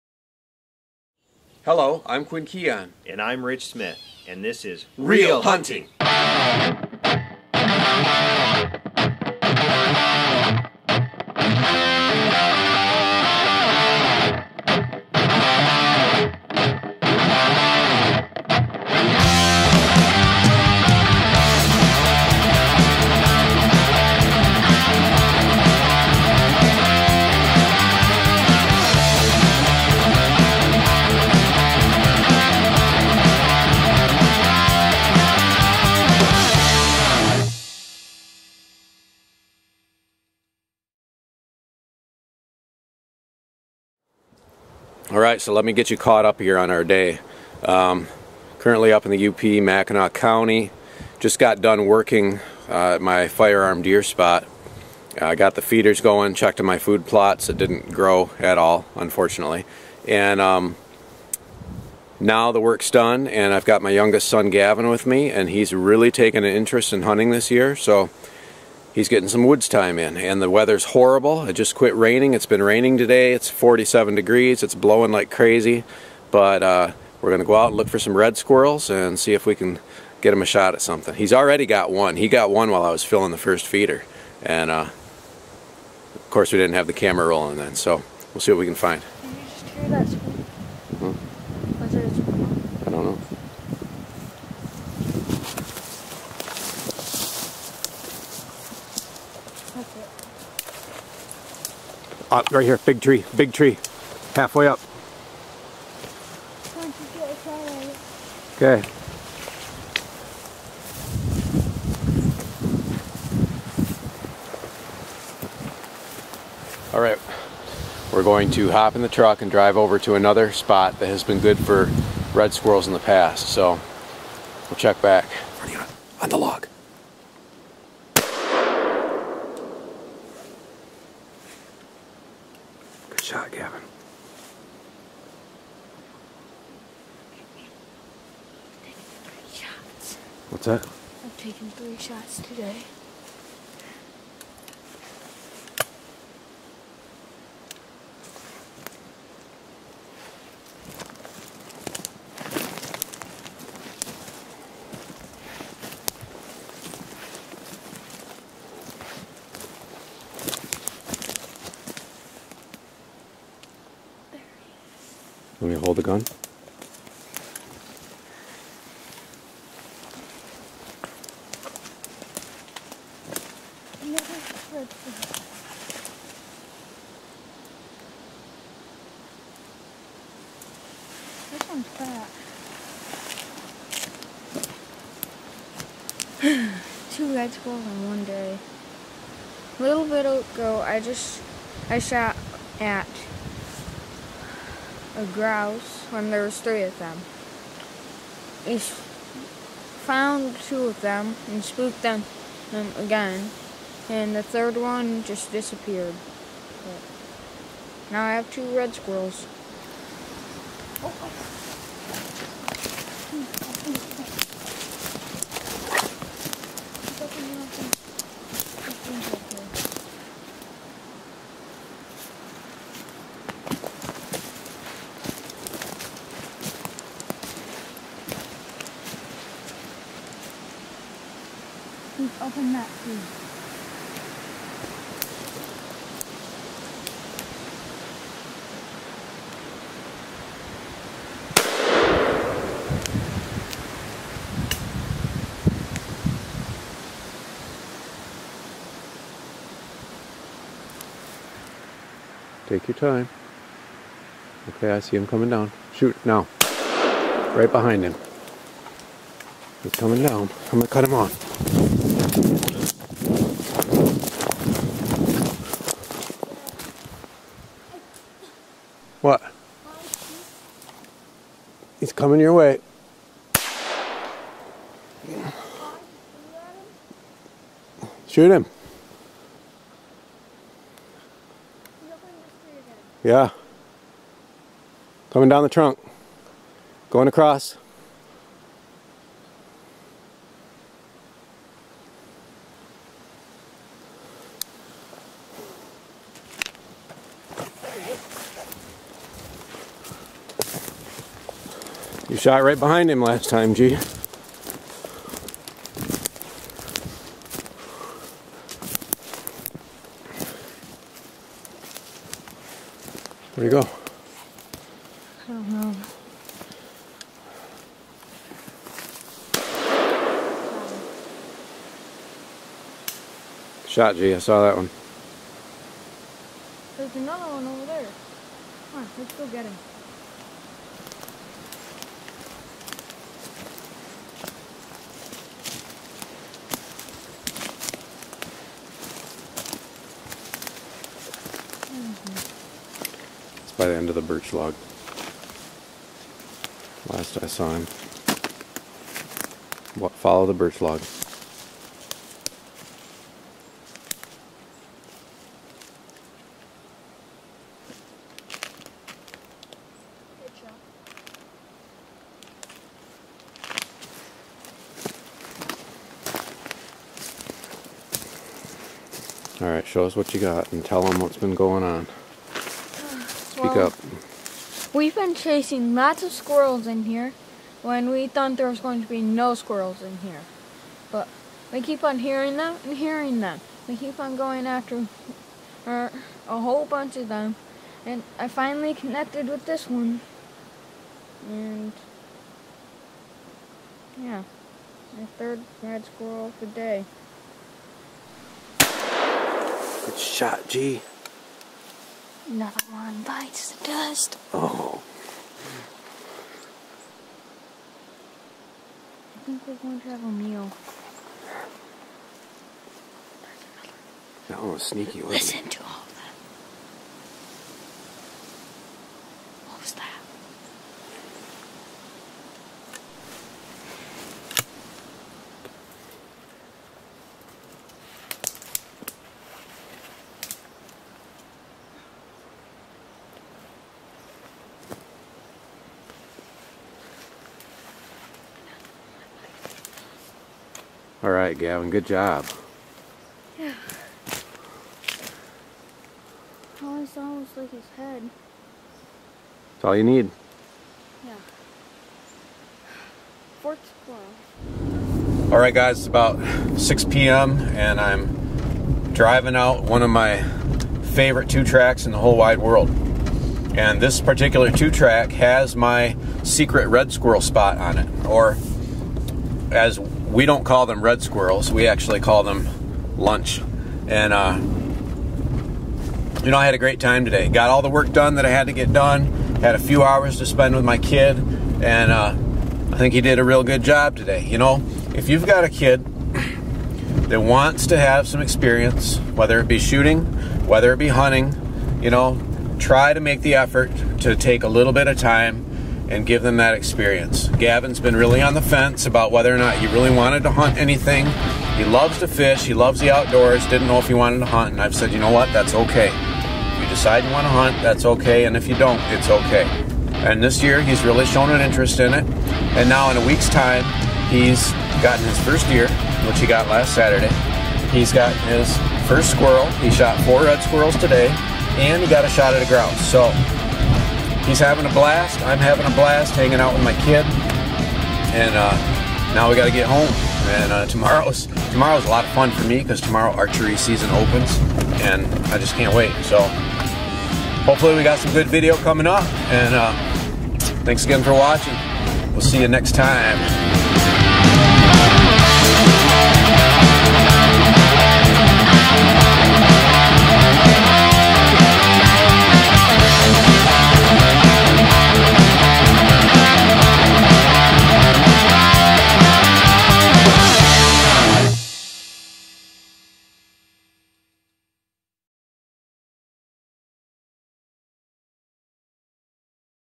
Michigan U.P. Youth Red Squirrel Hunting - .410
Having one day to fit it all in is often the way things play out.  Quinn and his son head to Mackinac County to do some work.  Once the work is done they head out into the big woods in search of red squirrels.